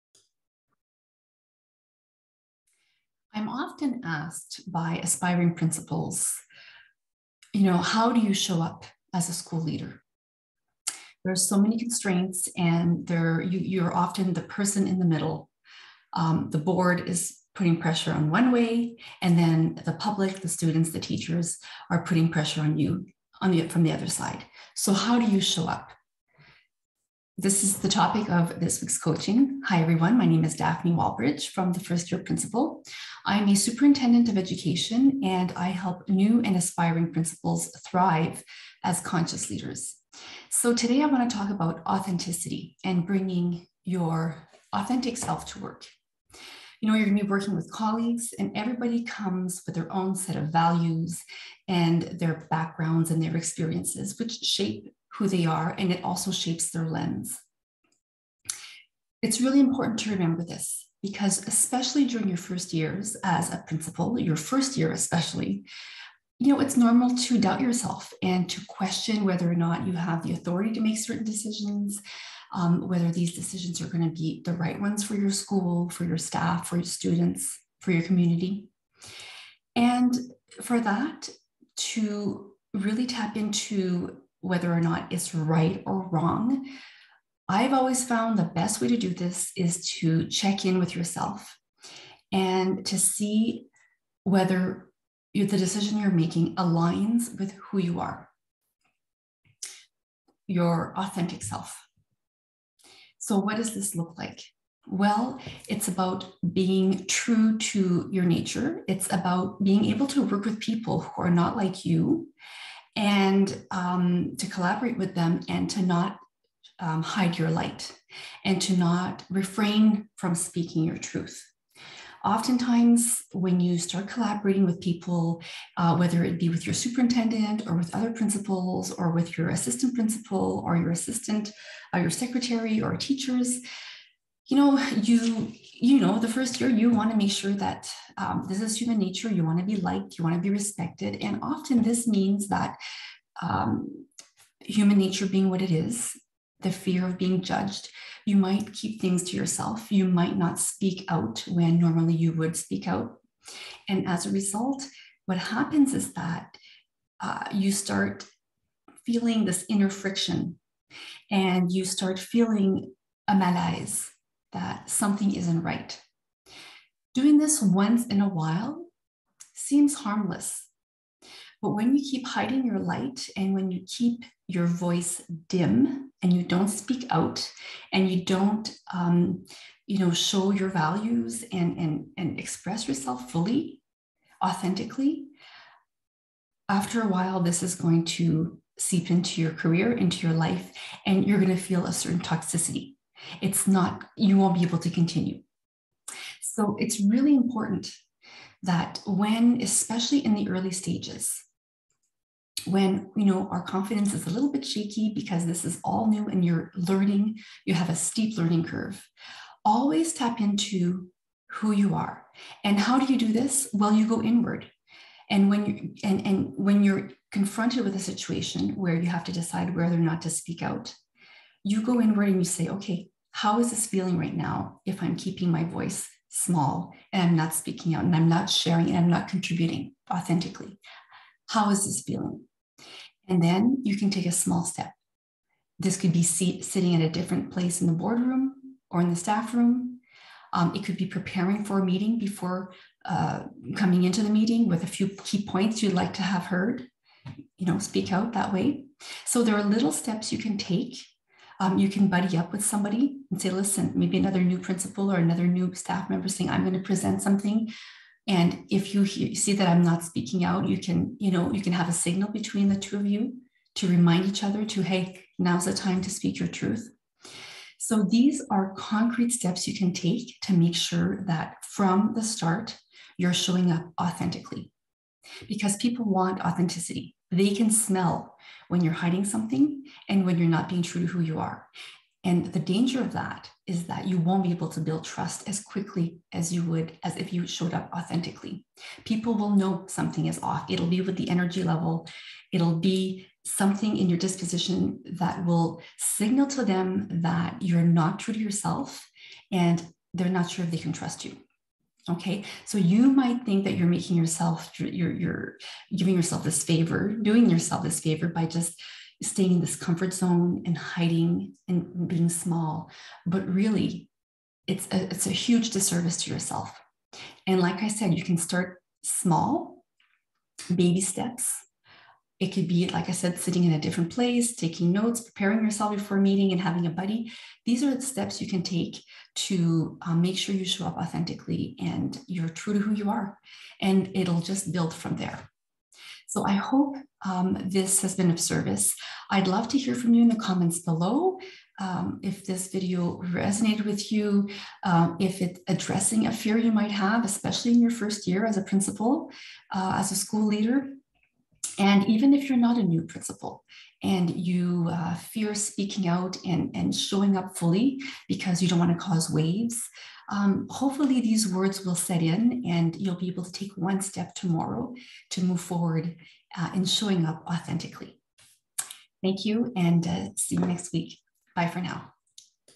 How to show up authentically and build trust
As a new school leader, you’ll face many new learning opportunities. This is a good thing! These moments will help you grow and find your voice. However, it’s important to remember that to help you find your leader’s voice; you also need to lean in even when you fear being judged or criticized. In this week’s coaching, I unpack authenticity and its role in building trust.

To get a copy of my book,